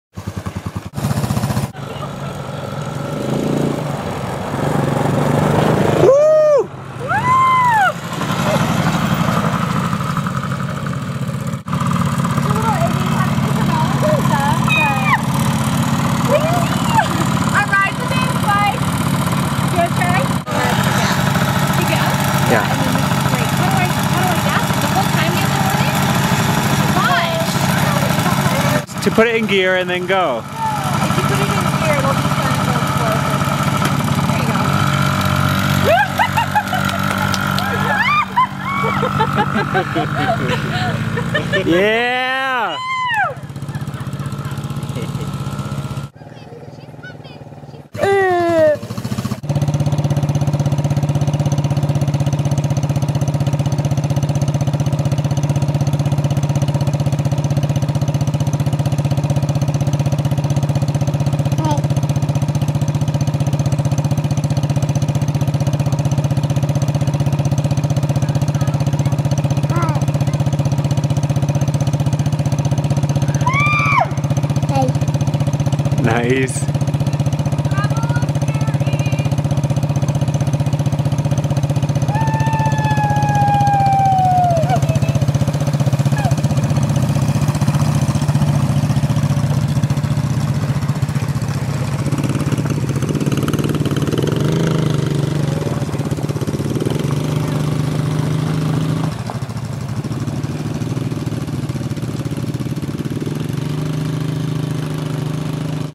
1stmoto
Jacove and Anne's 1st motorcycle ride.